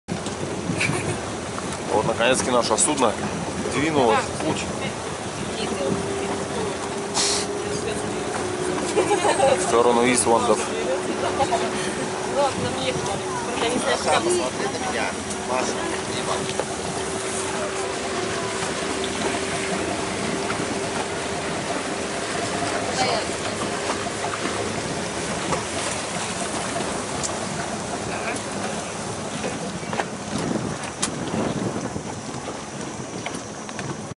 Lake pregnant virgins, sail from the island of Langkawi.
The excursion to Lake pregnant virgins, sail from the island of Langkawi.
Экскурсии на озеро беременной девы, отплываем от острова Лангкави.